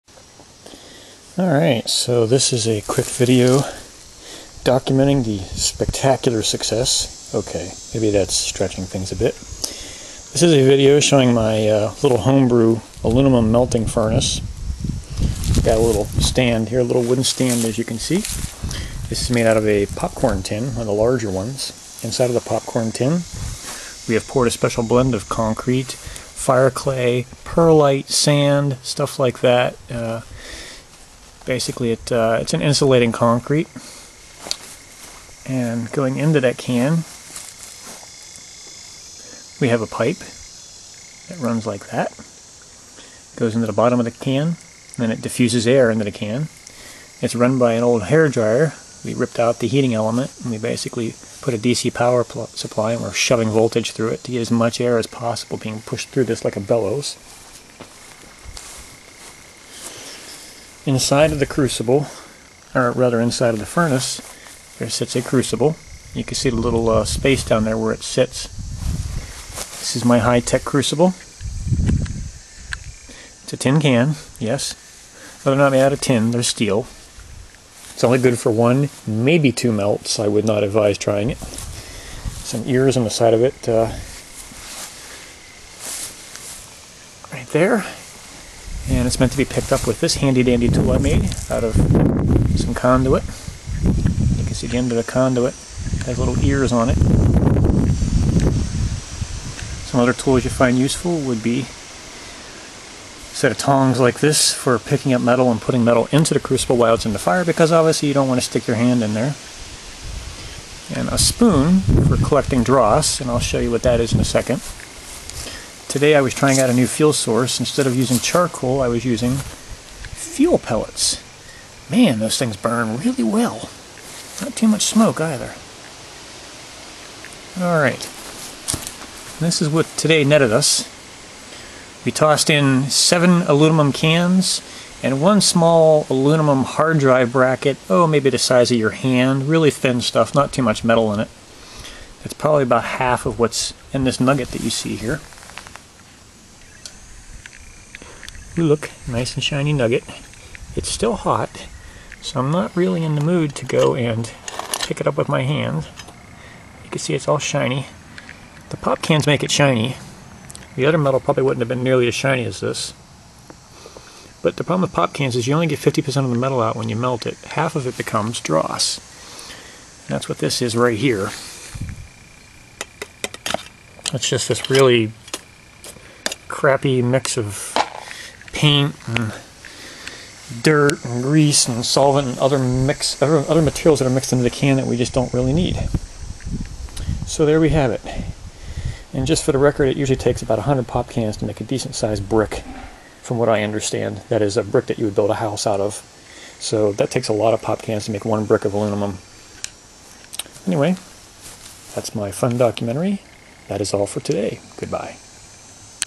Aluminum Furnace
A bare-bones, low-tech, backyard-style aluminum melting furnace. Dont try this at home, unless you do, in which case I say "Good Luck."

Took this vid after I had just melted some AL using a "new and improved" blower system (ie. a hairdryer with a bigger power-supply).